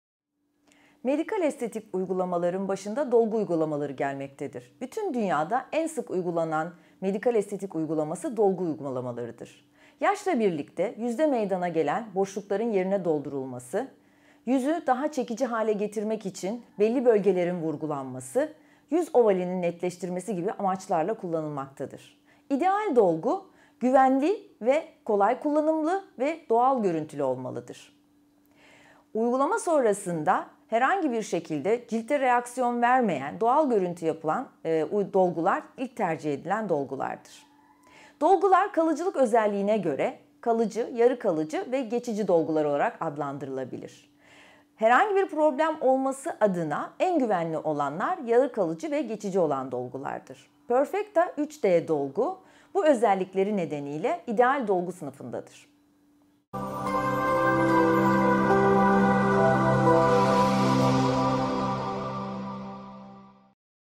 Dolgu Nedir ? İdeal Dolgu Maddesi Nasıl Olmalıdır ?
Dr. Ayşegül Girgin Dolgu Nedir ? İdeal Dolgu Maddesi Nasıl Olmalıdır ? Bilgilendiriyor.

Perfectha 3D Dolgu hakkında daha ayrıntılı bilgi almak için web sitemizi ziyaret edebilirsiniz.